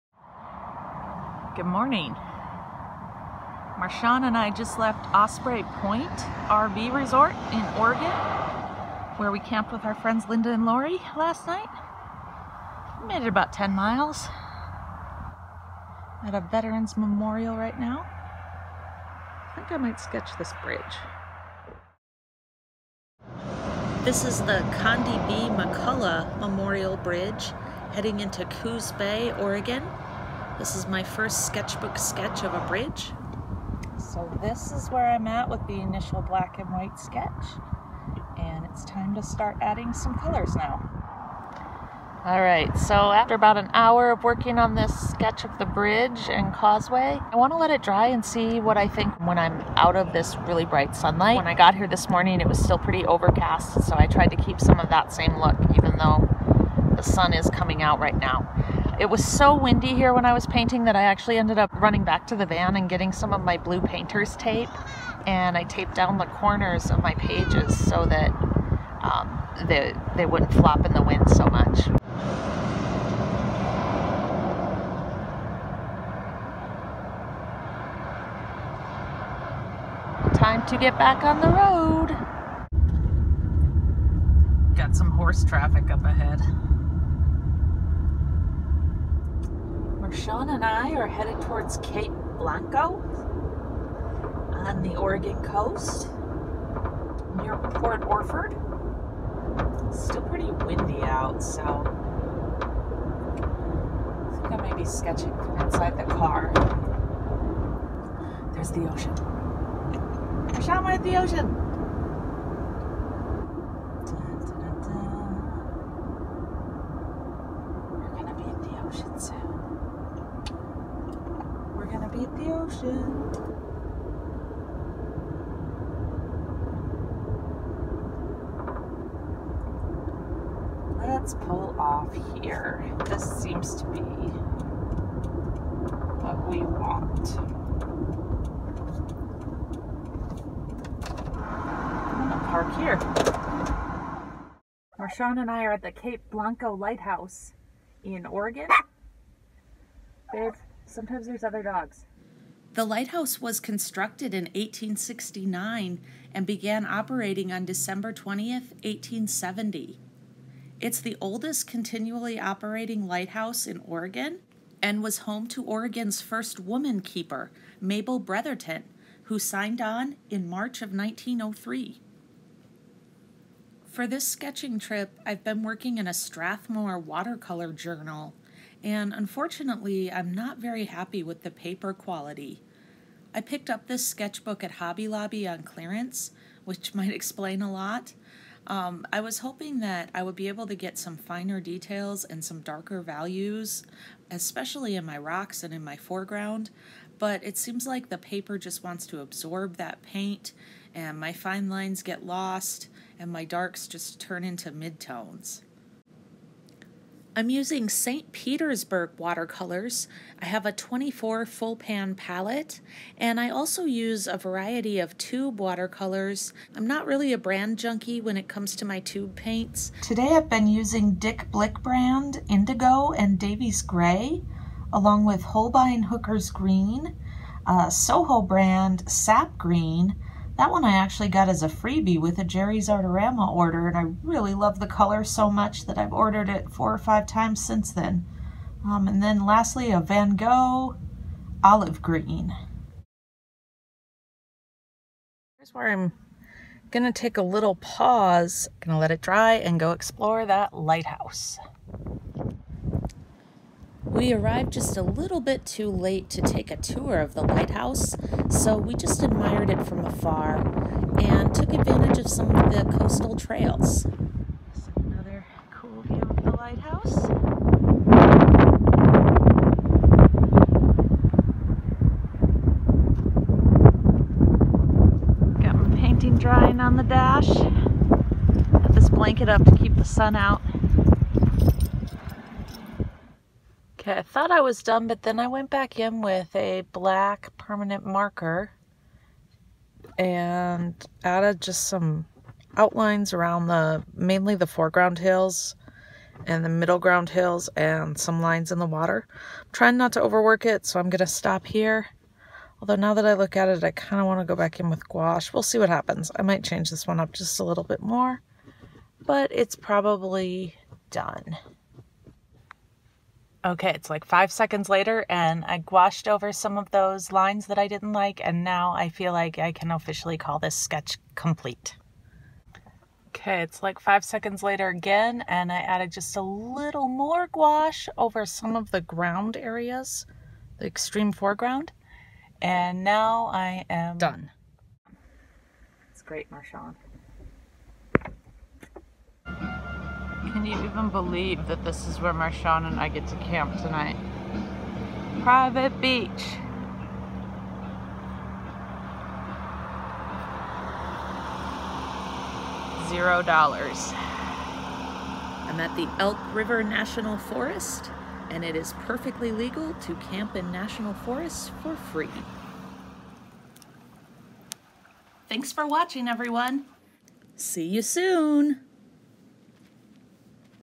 Watercolor sketching along the Oregon coast - Cape Blanco Lighthouse and Coos Bay Bridge
My first time sketching a bridge in my sketchbook... AND my first lighthouse sketch!

I’m on a painting adventure, traveling the west coast with my chihuahua Marshawn in our van Bonnie.

When we left Osprey Point this morning, I thought we’d get as far as our destination in California today. but it looks like we’ll be spending a few more (gorgeous) days in Oregon instead. I can’t say that I mind.